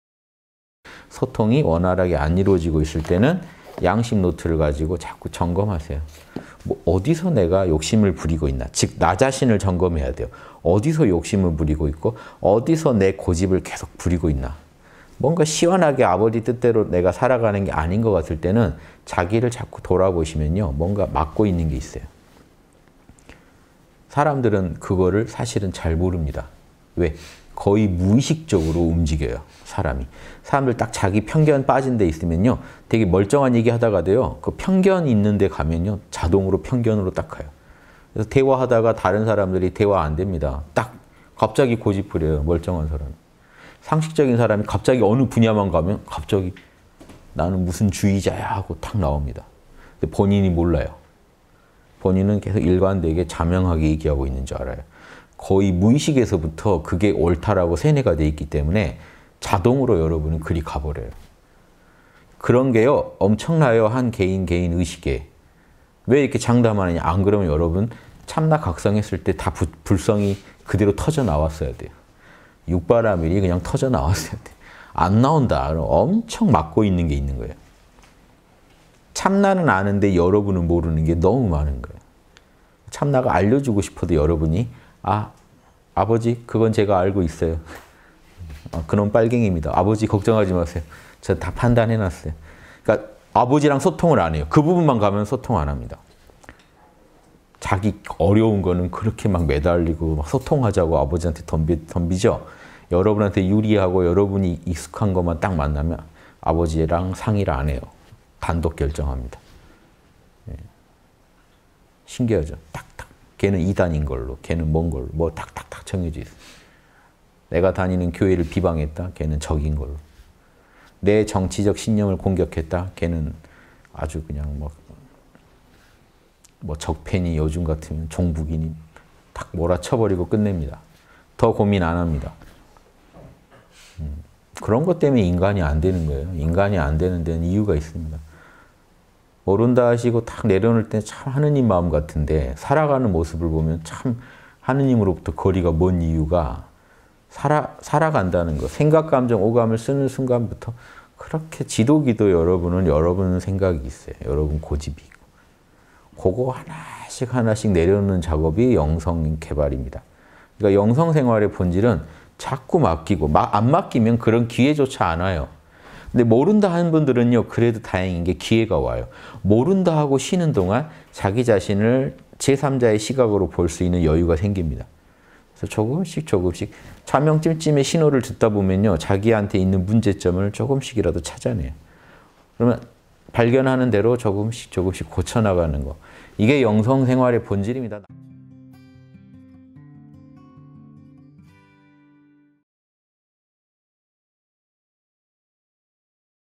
[3분 양심] 나를 막고 있는 엄청난 고집들_홍익학당.윤홍식
(170906 수밤 대담) 소통이 원활하게 안 이루어지고 있을 때는
양심노트를 가지고 자꾸 점검하세요.
어디서 내가 욕심을 부리고 있나?
즉 나 자신을 점검해야 돼요.

어디서 욕심을 부리고 있고, 어디서 내 고집을 계속 부리고 있나?
뭔가 시원하게 아버지 뜻대로 내가 살아가는 게 아닌 것 같을 때는,
자기를 자꾸 돌아보시면, 뭔가 막고 있는 게 있어요.
사람들은 그걸 사실은 잘 모릅니다.
왜? 거의 무의식적으로 움직여요,사람이.

사람들 딱 자기 편견 빠진 데 있으면,
멀쩡한 얘기하다가도 편견 있는 데 가면 자동으로 편견으로 딱 가요.
대화하다가 다른 사람들이 대화 안 됩니다.
딱 갑자기 고집 부려요, 멀쩡한 사람이.
상식적인 사람이 어느 분야만 가면 갑자기
나는 무슨 주의자야 하고 탁 나옵니다.

본인이 몰라요.
본인은 계속 일관되게 자명하게 얘기하고 있는 줄 알아요.
거의 무의식에서부터 그게 옳다고 세뇌가 되어 있기 때문에
자동으로 여러분은 그리 가버려요.
그런 게 엄청나요, 한 개인 개인의 의식에.

왜 이렇게 장담하느냐?
안 그러면 여러분 참나 각성했을 때
불성이 그대로 터져 나왔어야 돼요.
육바라밀이 그냥 터져 나왔어야 돼요.
안 나온다 그러면 엄청 막고 있는 게 있는 거예요.

참나는 아는데 여러분은 모르는 게 너무 많은 거예요.
참나가 알려주고 싶어도 여러분이
‘아! 아버지! 그건 제가 알고 있어요.’
‘그놈 빨갱이입니다.’
‘아버지! 걱정하지 마세요, 저 다 판단해놨어요.’
아버지랑 소통을 안 해요. 그 부분만 가면 소통을 안 합니다.

자기 어려운 건 그렇게 매달리고,
소통하자고 아버지한테 덤비지요?
여러분한테 유리하고 여러분이 익숙한 것만 딱 만나면,
아버지랑 상의를 안 해요. 단독 결정합니다.
신기하죠? 그것은 이단인 걸로,
그것은 뭔 걸로, 탁 탁 탁 정해져 있어요.
내가 다니는 교회를 비방했다? 그것은 적인 걸로.
내 정치적 신념을 공격했다? 그것은 아주 그냥..
적폐니 요즘 같으면 종북이니 탁 몰아쳐버리고 끝냅니다.
더 고민 안 합니다.

그런 것 때문에 인간이 안 되는 거예요.
인간이 안 되는 데에는 이유가 있습니다.
‘모른다’ 하시고 탁 내려놓을 때는 참 하느님 마음 같은데,
살아가는 모습을 보면 하나님으로부터 거리가 먼 이유가,
살아간다는 것, 생각 감정 오감을 쓰는 순간부터,
그렇게 지독히 여러분은 여러분의 생각이 있어요, 여러분 고집이.

그것 하나씩 하나씩 내려놓는 작업이 영성 계발입니다.
영성 생활의 본질은 자꾸 맡기고,
안 맡기면 그런 기회조차 안 와요.
모른다하는 분들은 그래도 다행인 게 기회가 와요.
모른다하고 쉬는 동안 자기 자신을,
제3자의 시각으로 볼 수 있는 여유가 생깁니다.

조금씩 조금씩, 자명 찜찜 신호를 듣다 보면,
자기한테 있는 문제점을 조금씩이라도 찾아내요.
그러면 발견하는 대로 조금씩 조금씩 고쳐나가는 것,
이게 영성 생활의 본질입니다.